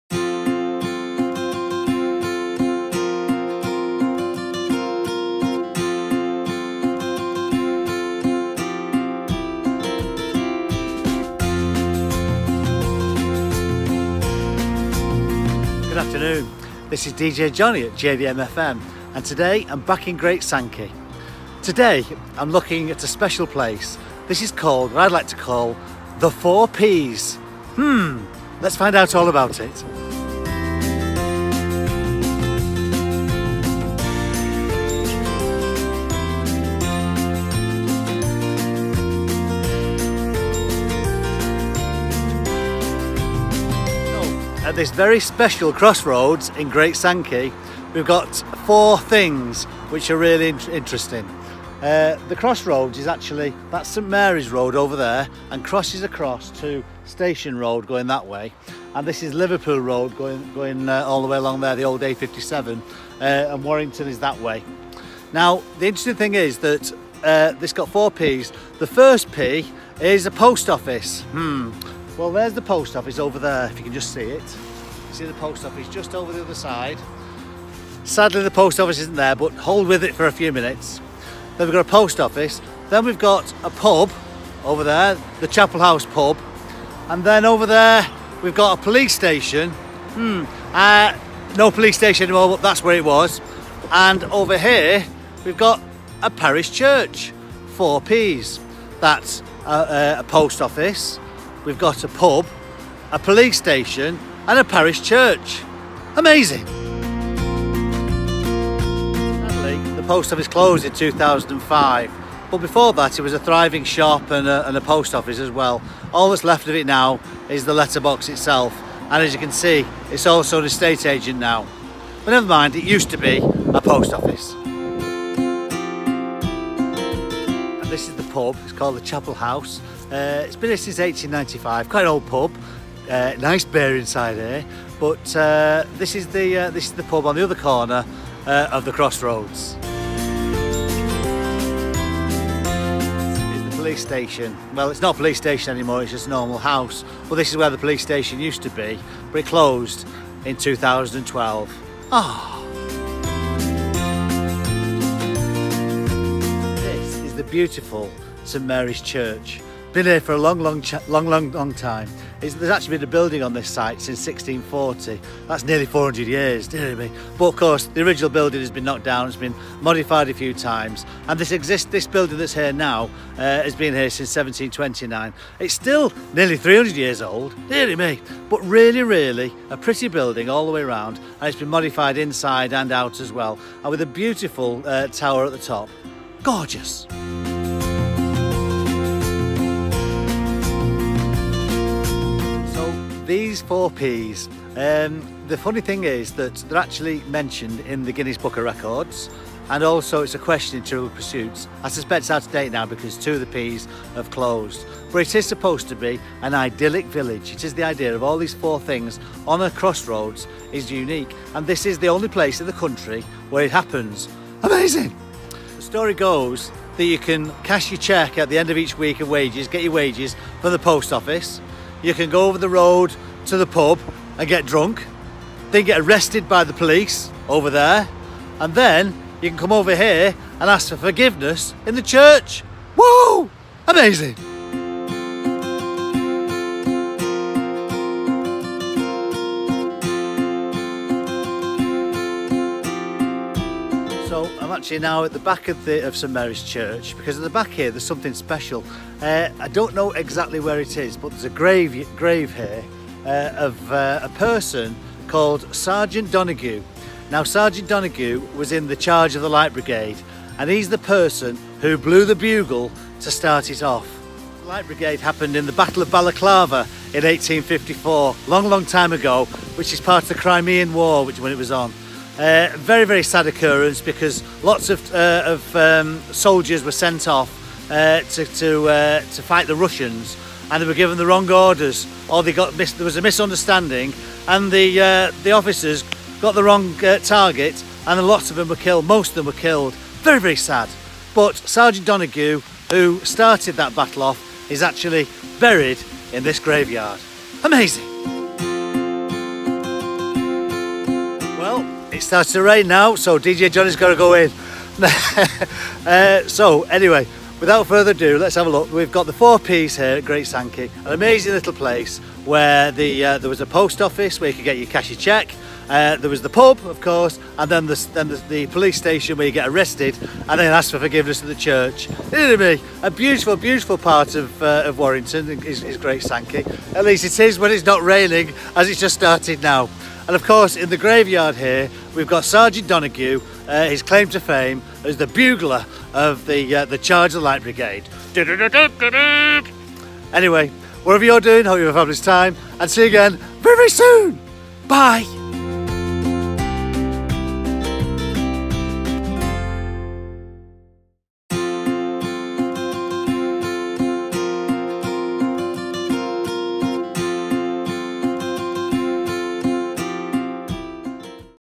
DJJohnny visits the 4 Ps at Gt Sankey!
DJJohnny returns to the Penketh and Great Sankey areas, with his continued history of Warrington! This time looking at “The 4 Ps”! Visiting, what was, the last remaining crossroads in the country - that has a (or had) a post office, a pub, a police station and a parish church!